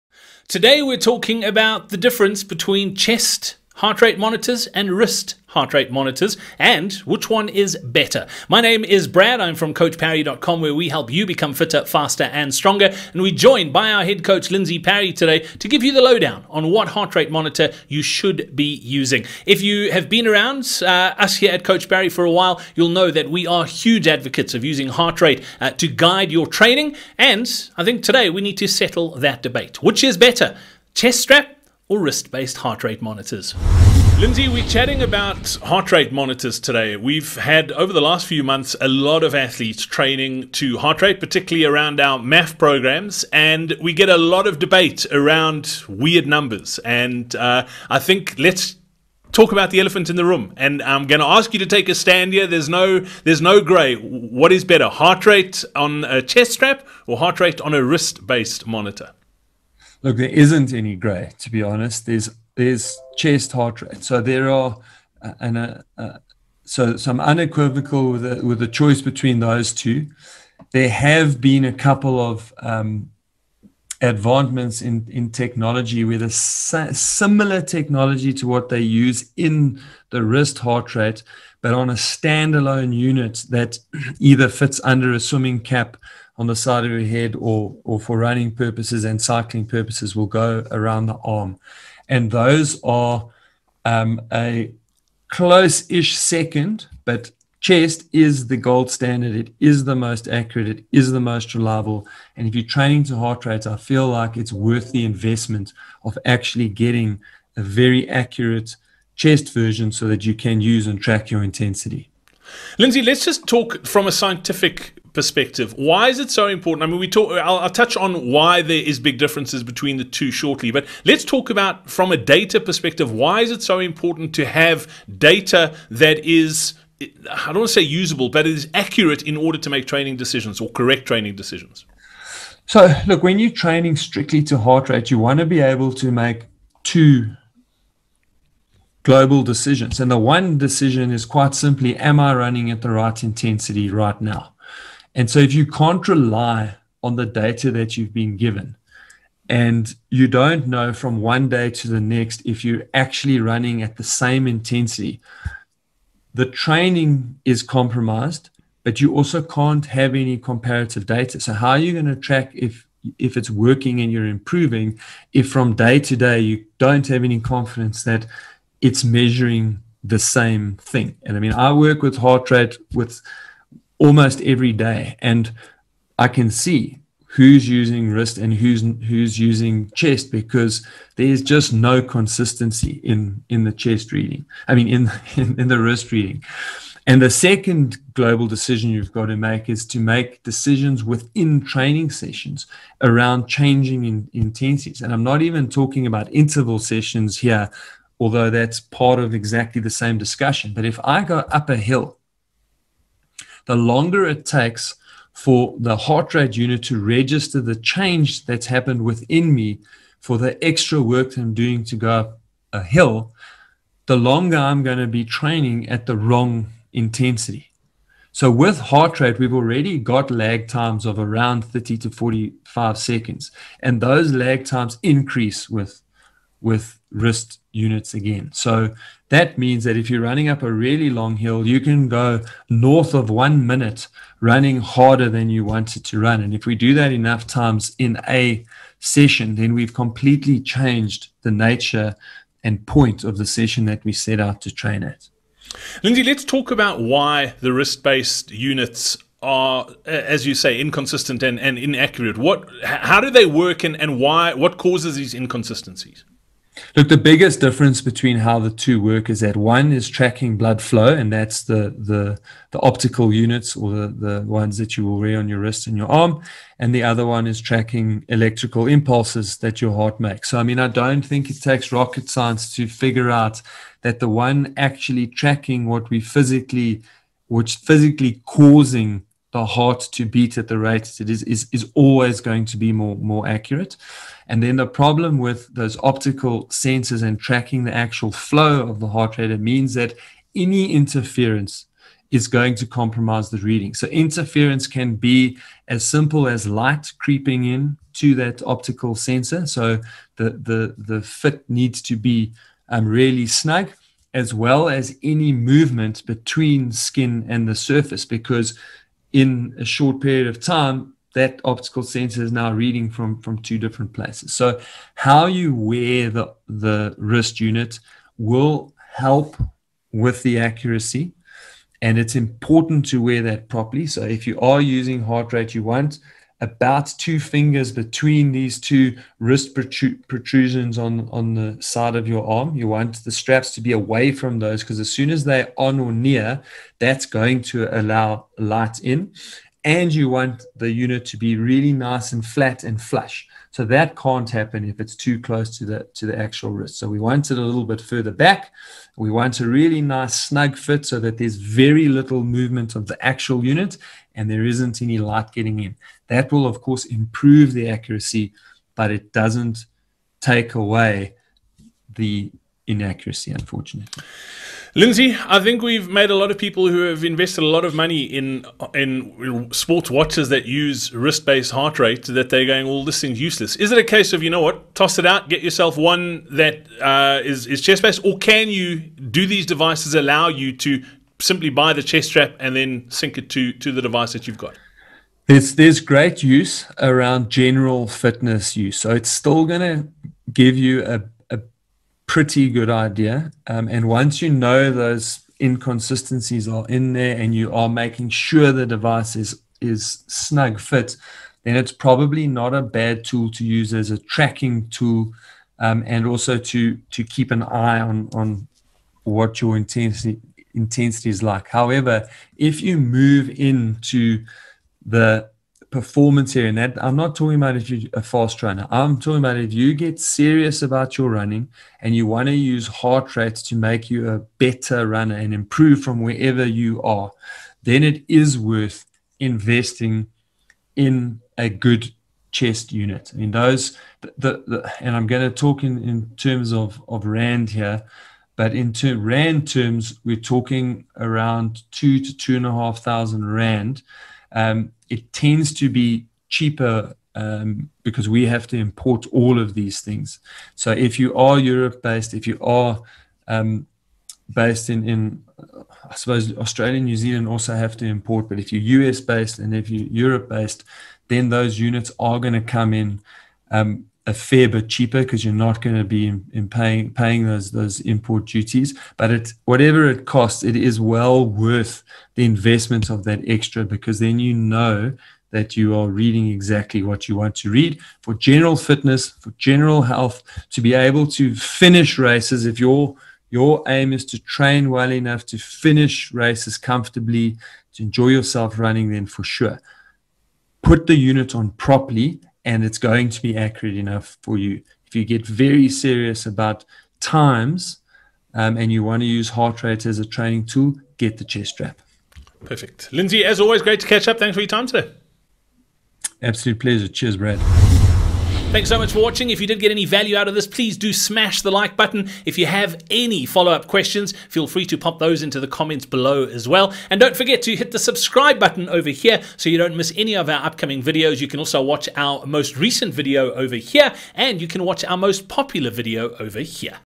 Which Heart Rate Monitor Is The MOST Accurate?
Heart rate training is one of the best methods of monitoring your training progress. But there are so many tech options out there to choose from to aid you in your training in particular surrounding the use of wrist-based heart rate measuring or chest strap based measurements. But which is the best? Find out here...